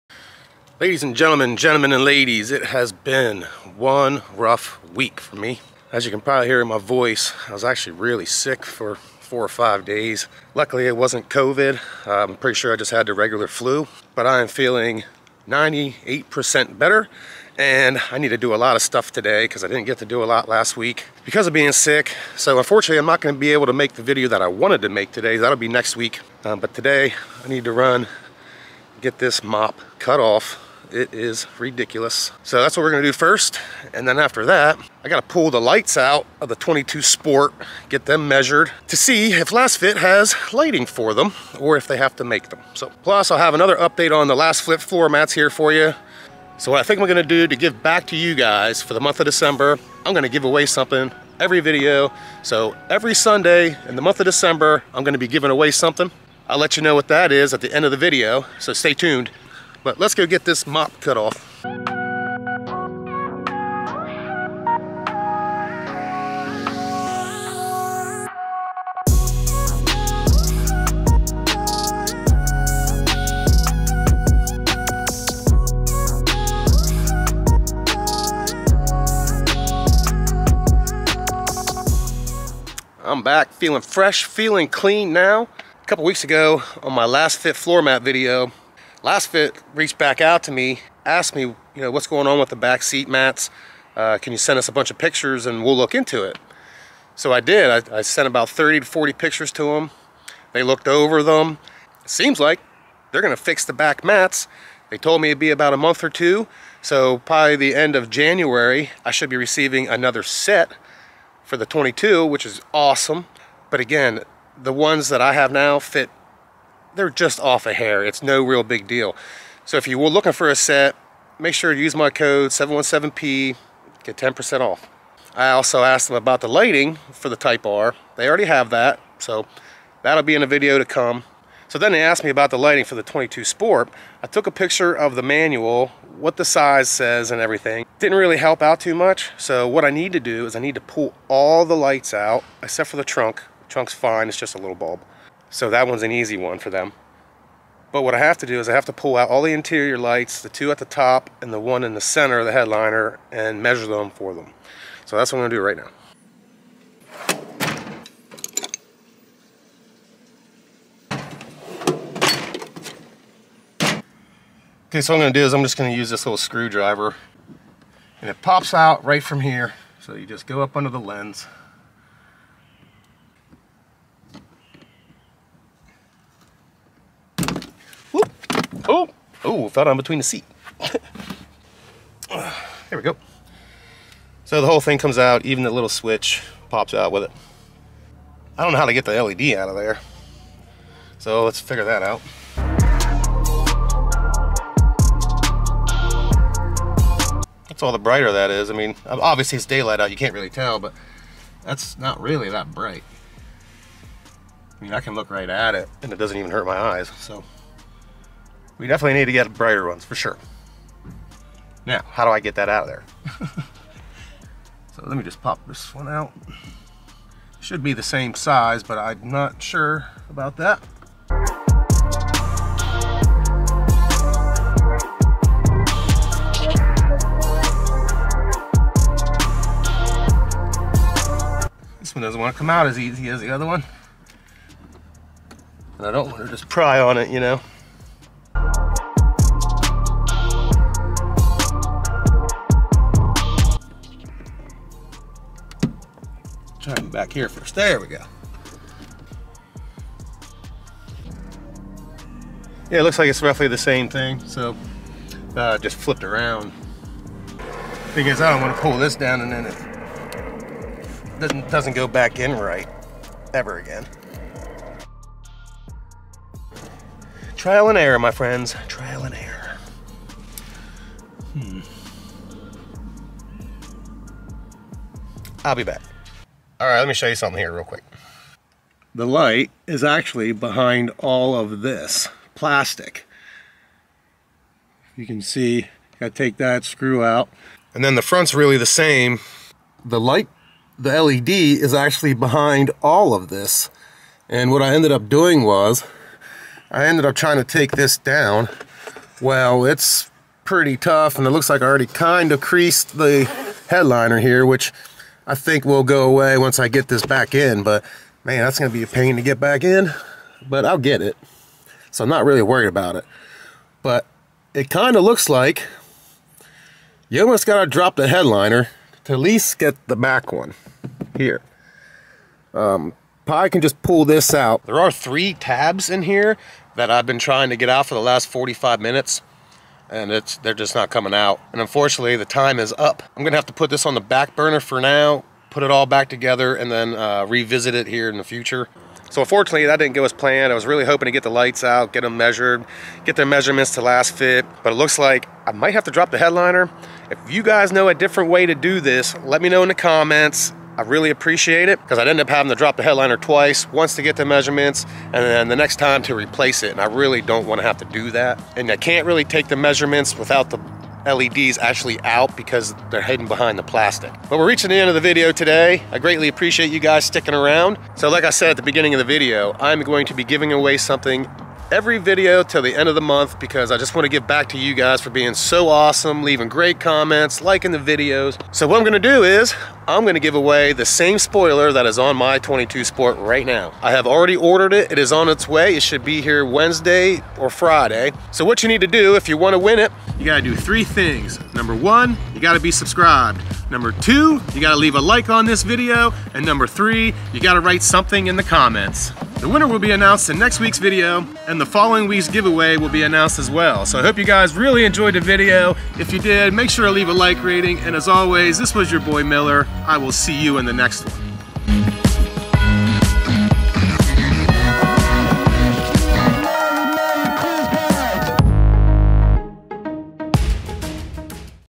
11th Gen 2022 Honda Civic interior light removal, didn't go as planned!
Not the video I had planned for this week, being sick really sucks!. I have an update on the 2022 Civic floor mats from Lasfit. You know what to do if you enjoyed! Thank you to all who participated!

link to spoiler